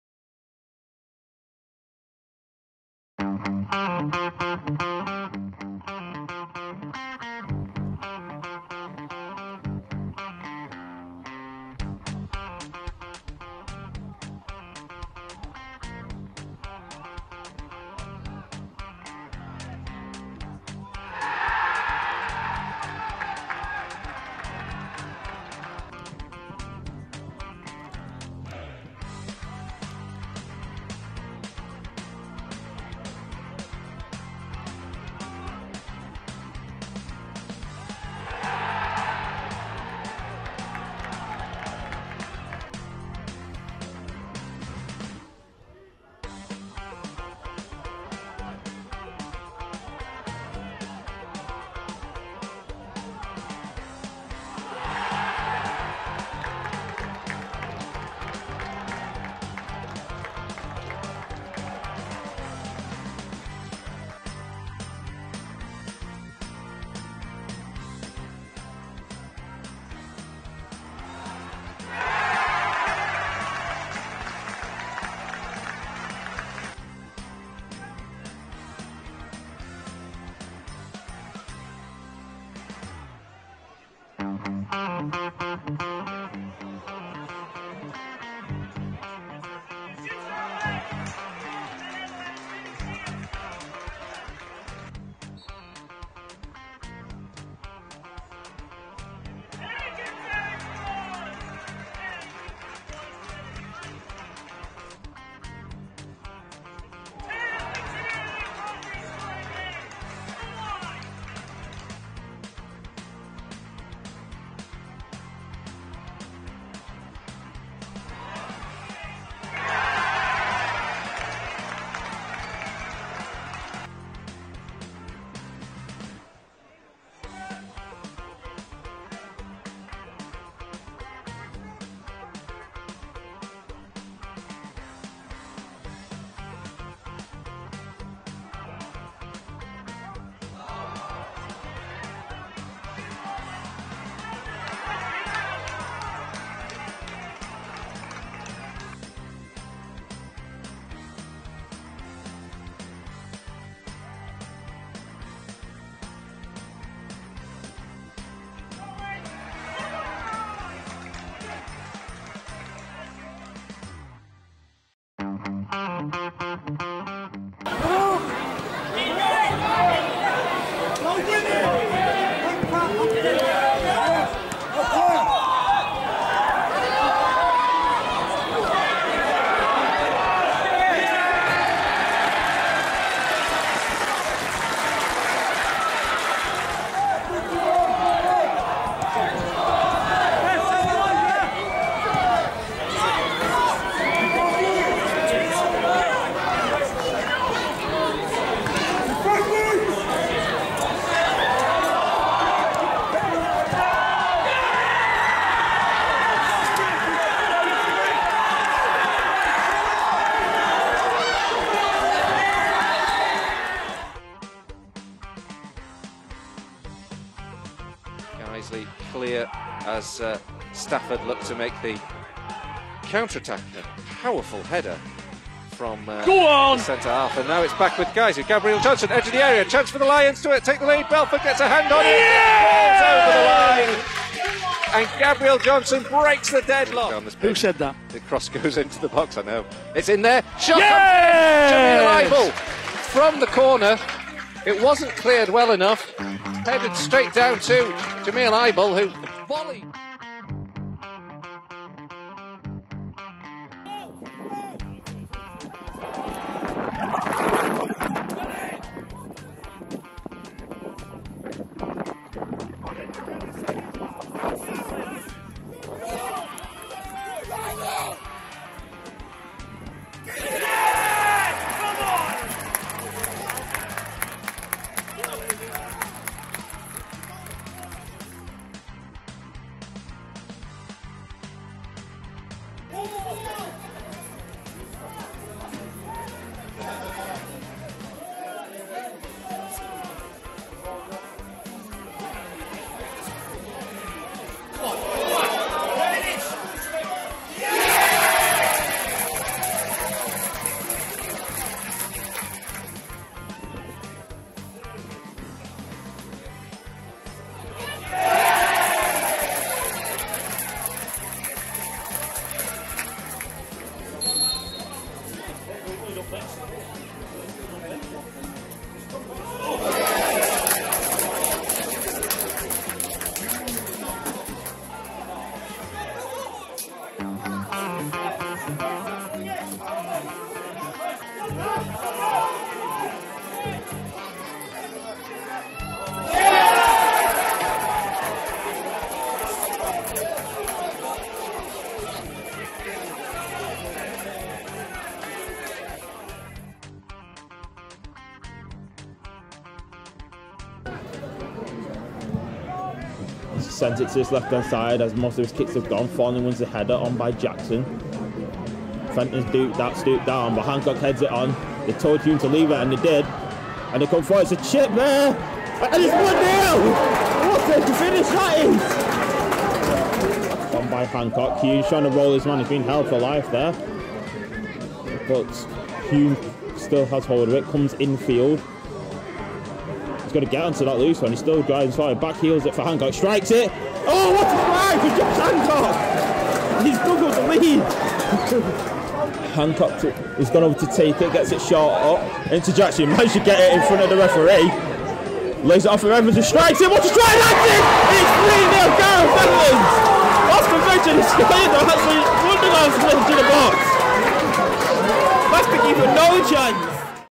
2023/24 NPL Goal Show: Episode 7
Welcome to the latest episode of the NPL Goal Show!

Watch all the action from the latest round of fixtures as Radcliffe extended their position at the top of the table with a victory at home. Meanwhile, Morpeth Town came away with all three points after an impressive performance away at FC United of Manchester, while Marske United claimed an important win in a bottom-of-the-table clash against Basford United.